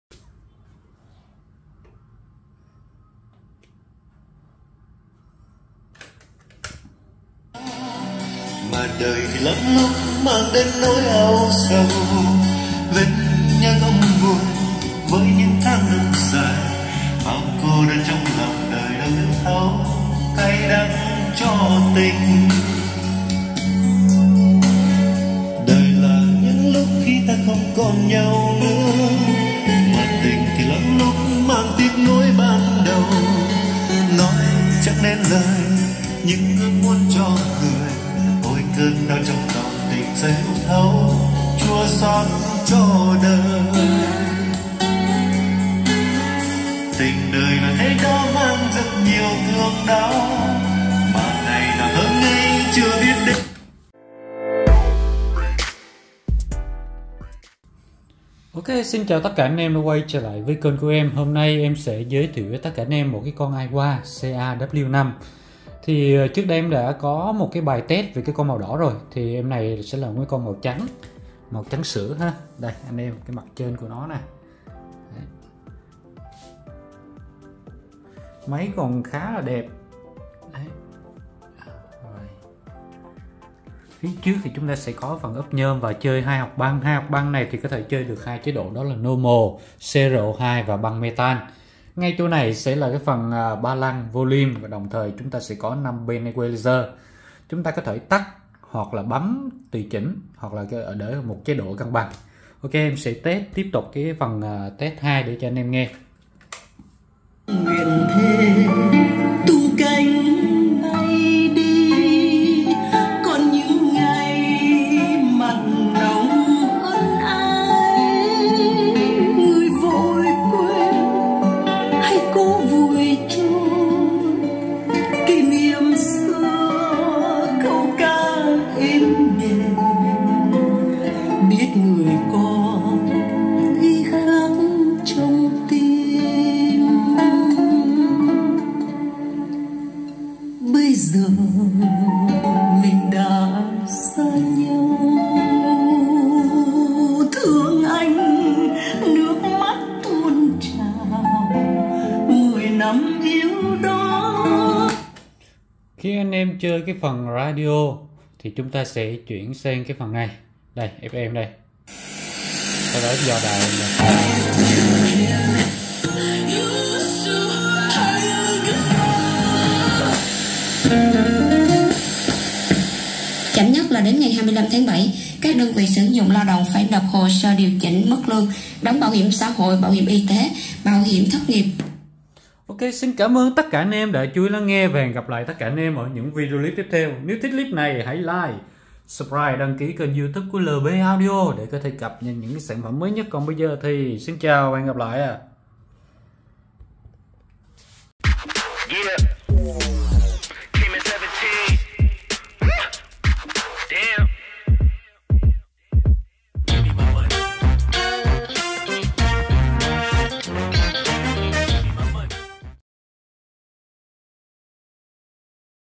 Cassette AIWA CA-W5 năm 1984 với chất âm Sáng giá 6tr|| LP AUDIO
Ở modle CA-W5 này Aiwa khác đa dạng về màu sắc, từ các màu căn bản như trắng, đen đỏ còn có cả màu xanh. Cùng với sự da dạng và chất âm mà nó mang lại, Aiwa CA-W5 (Aiwa W50) là model phổ biến năm 1984
Giao lưu 6tr
Thiết kế dạng cassette 3 cục rời cổ điển vuông vức nam tính, Đài Aiwa CA-W5 có đầy đủ các tính năng mà ng chơi cần
- Trang bị 2 hộc băng cassette
- Radio full tần số bắt hết đài Việt Nam
- Tùy chỉnh âm thanh với 5 Band EQ theo sở thích người dùng
- Chế độ ghi DNR như thu từ cửa băng 1 sang 2, thu tốc độ nhanh…
- Sử dụng 3 loại băng Normal, metal, và Cro2
- Cổng Line kết nối với điện thoại
- Có cổng mic và cỏng tai nghe
- Kích thước máy 580 x 158 x 158 mm
- Đài cassette AiwaW5 Nặng 5.8 kg
-  Made in Japan, điện 100v, nguồn DC 12V và sử dụng 8 cục pin

Kết bạn Zalo để thêm thông tin sản phẩm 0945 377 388
thông tin giá bán và video test tại youtube: